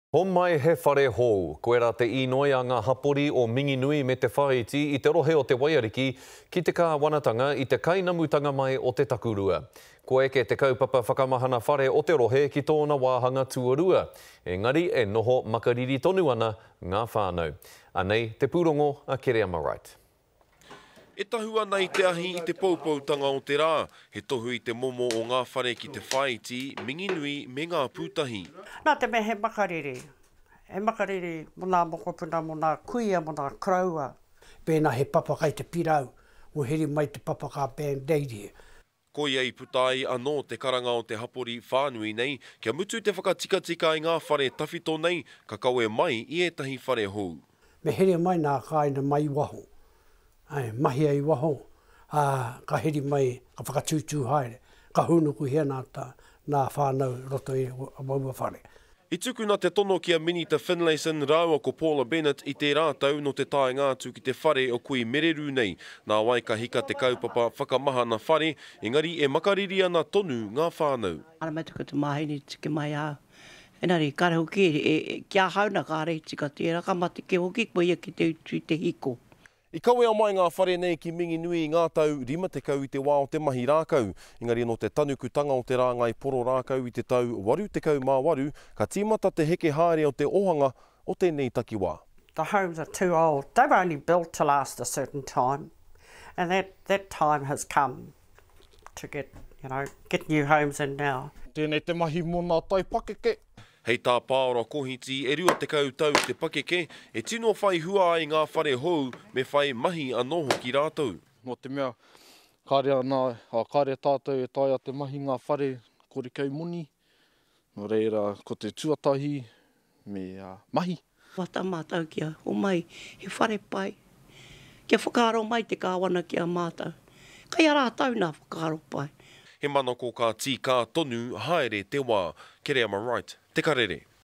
Minginui residents face a harsh winter in 50-year-old homes
As winter approaches, residents of the Minginui-Te Whāiti district in the Bay of Plenty are pleading the Government to replace their 50-year-old houses. The insulation programme in the district is now in its second phase, but whānau are still battling the cold.